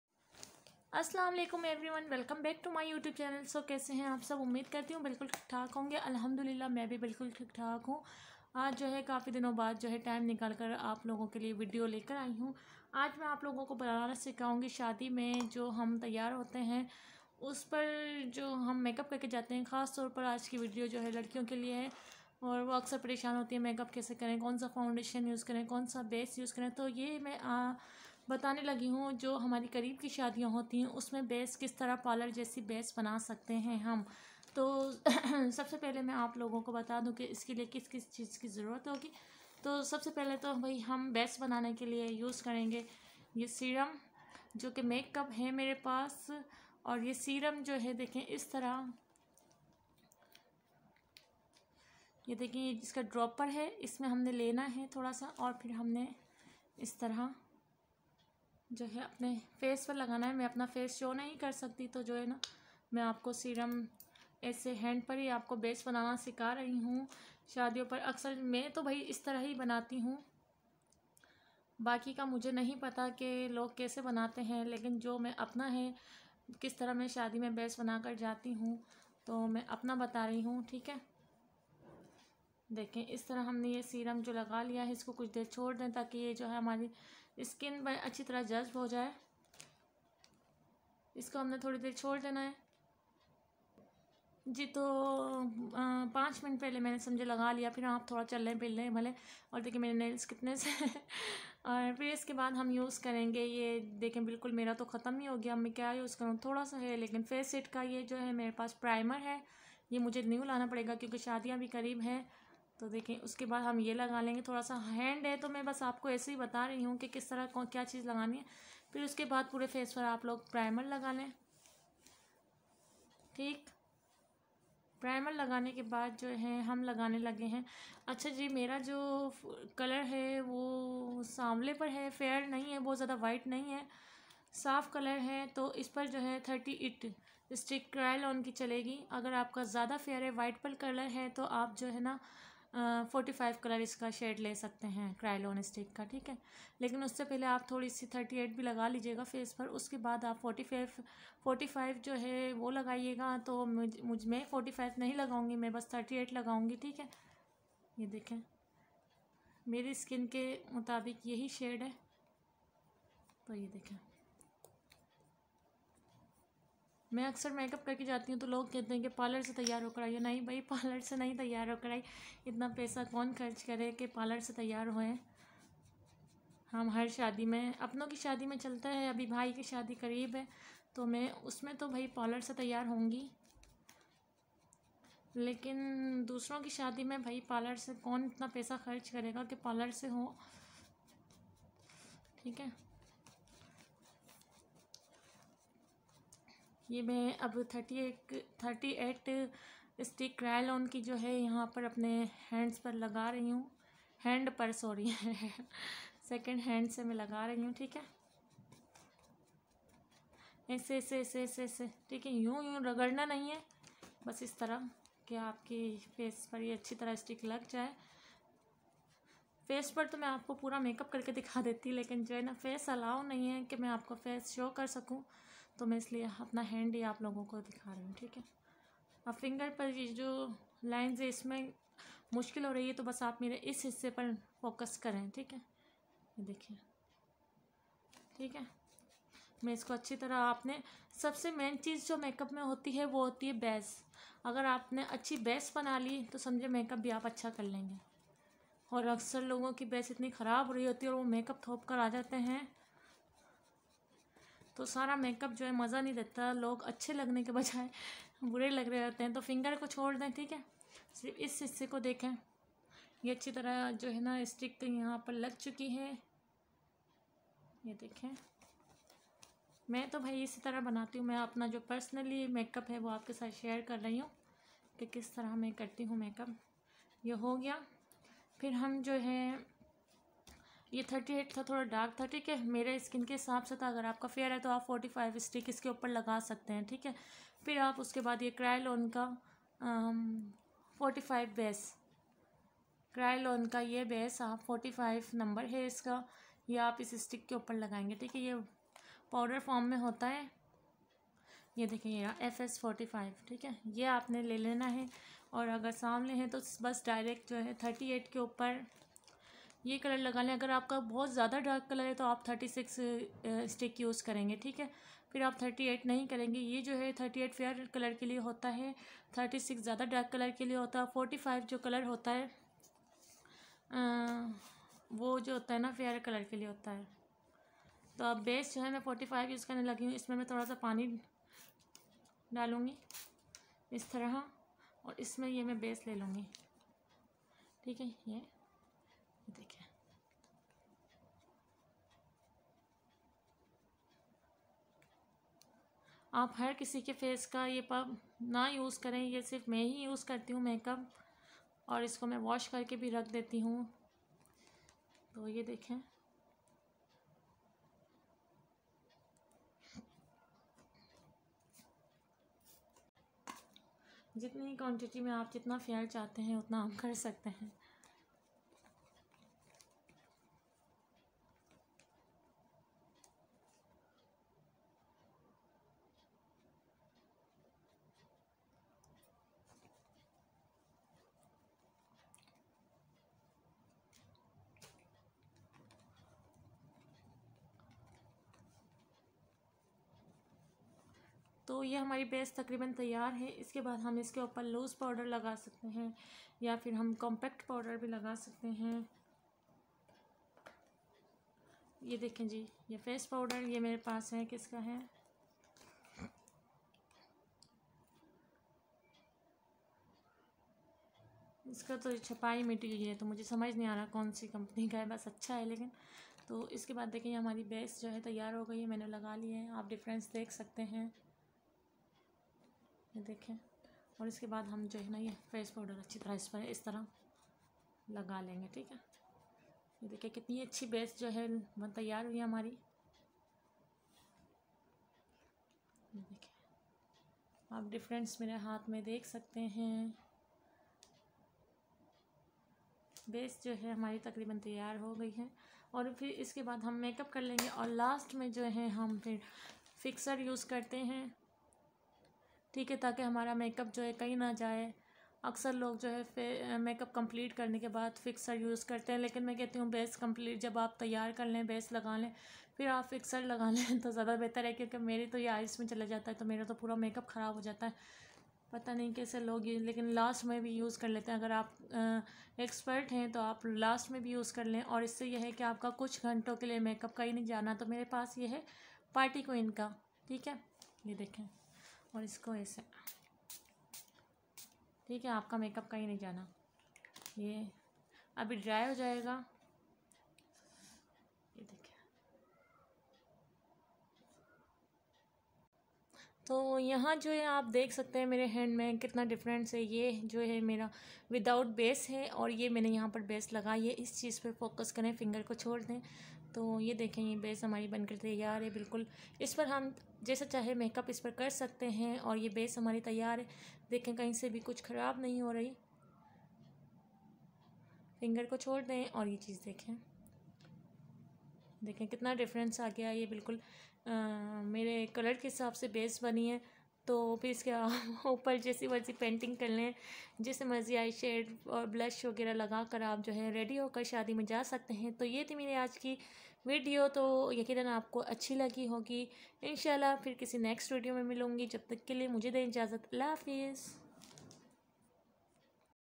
how to make parlour secret base at home | water proof base |best base in pakistan | ribs all rounder
hello everyone In today's video I will show you how you can make a base like a parlor sitting at home, best base in Pakistan and India, water proof base. parlor secret base.